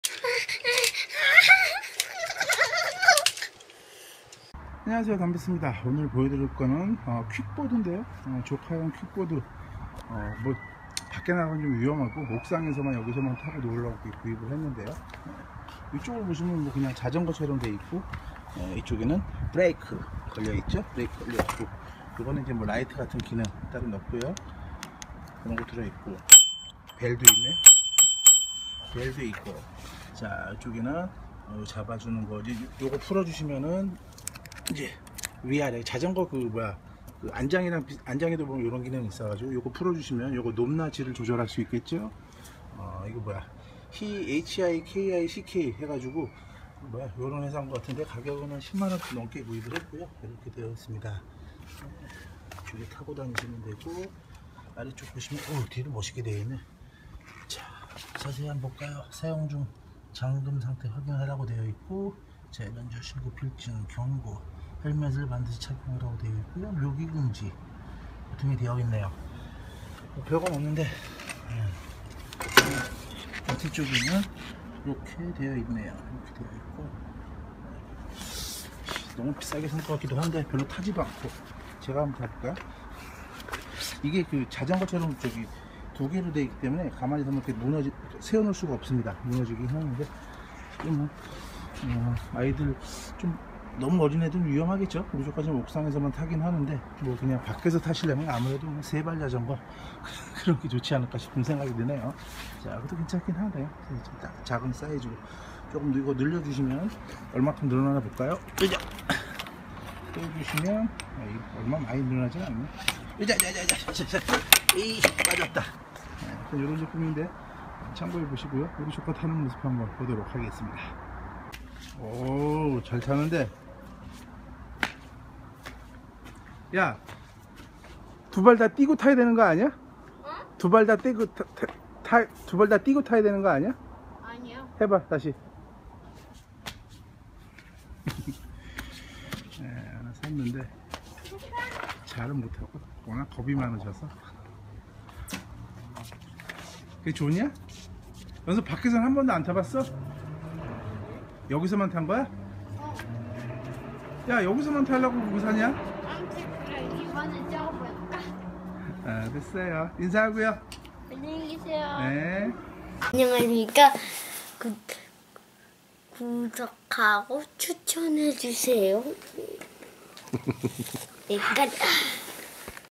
HIKICK 애니메이트 하이킥 아동 전용 킥보드, 아이들이 간단하게 타는 하이킥보드 소개
조카가 완구점에 가서 10만원이 좀 안되는 가격에 구입한 하이킥 퀵보드라는 제품인데, 아직은 어려서 옥상에서만 타고 놀고 있는데, 공원이나 놀이터에서 타고 놀기에는 좋을듯 한데, 자전거와 비슷하게 브레이크, led 점멸등, 벨 등도 달려있고, 높낮이 조절도 가능합니다.
다만 두발 방식의 바퀴가 달려있어서 그냥 서지는 못하고, 다른 발로 중심을 잡아야 해서 어린 아이에게는 좀 위험할수도 있어서 가급적 안전모를 꼭 쓰는게 좋을듯 한데, 참고해 보시기 바라겠습니다~